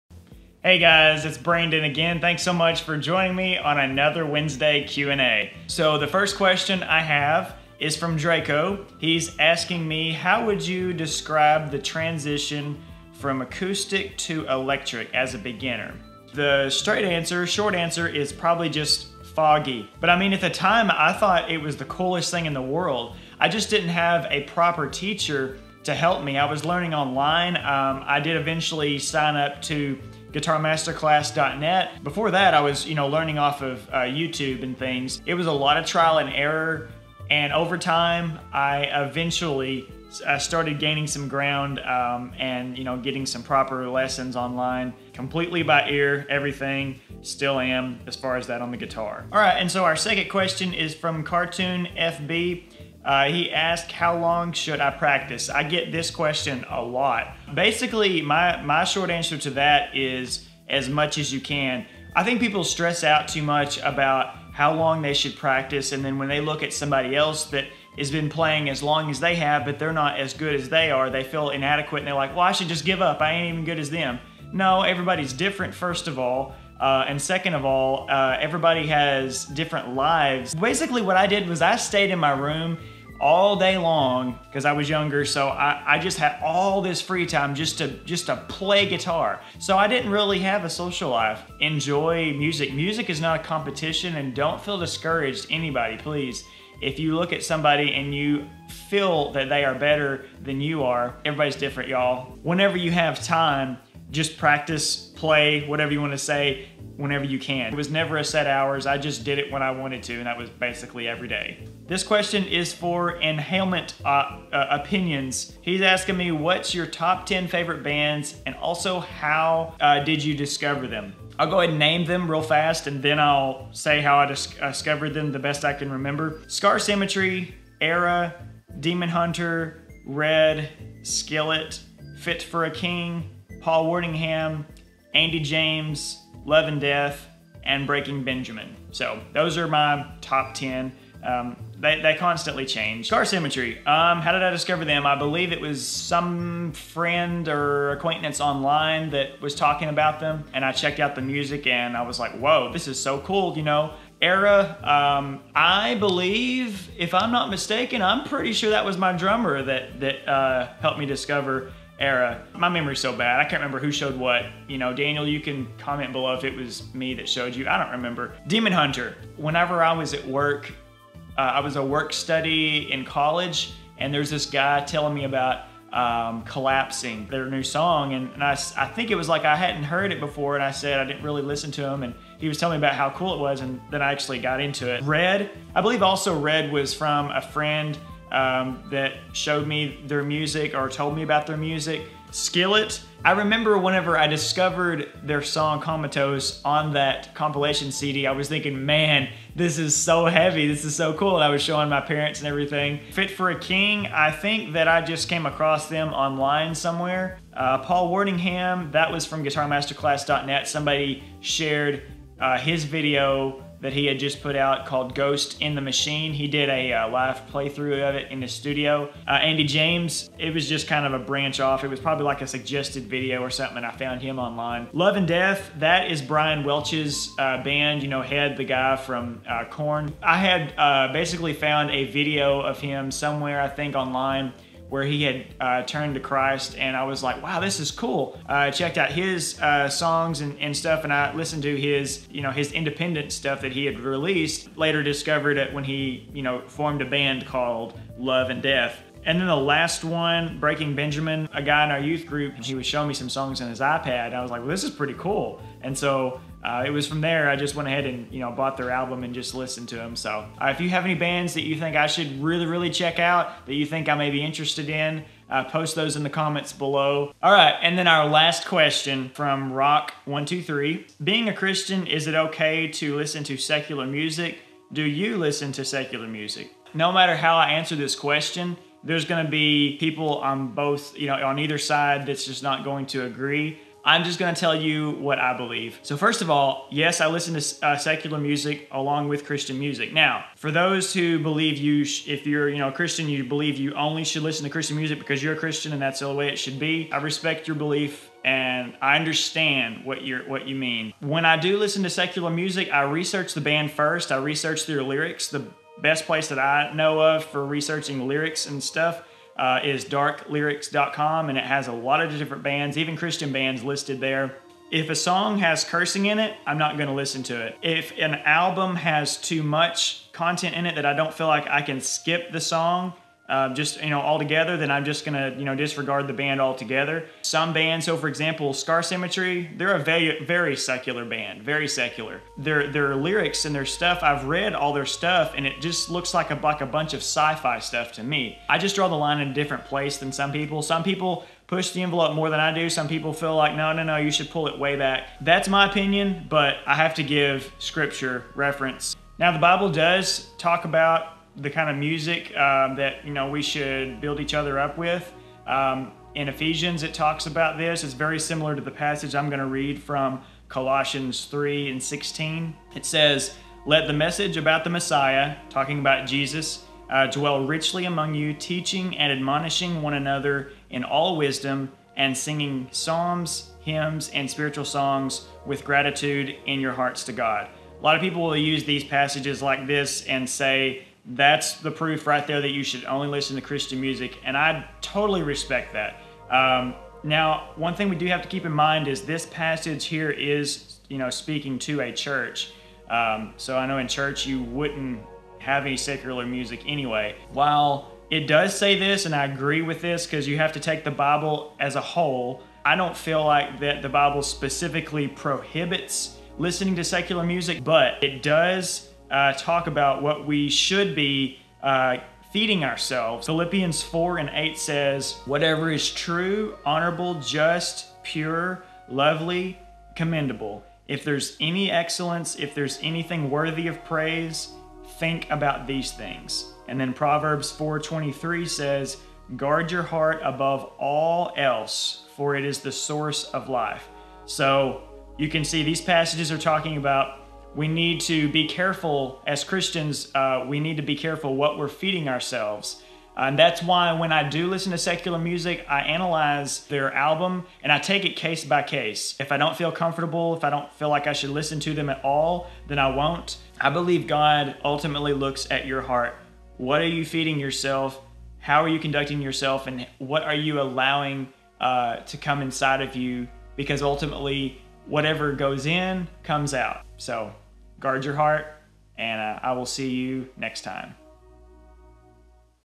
Q&A | Should Christians Listen to Secular Music?
This Q&A features some music questions I got from my fans recently. The last question is where I answer what's in the title.

Summary:
While I don't believe the Bible specifically prohibits listening to Christian music, it does instruct us to lift each other up in spiritual songs. It also says to guard our hearts in Christ Jesus because our hearts determine the course of our life. So my belief is that if you choose to listen to secular music, a Christian should be careful to pay attention to its message - ultimately where it leads your heart.
Scriptures referenced in this video:
Ephesians 5:19
Colossians 3:16
Philippians 4:8
Proverbs 4:23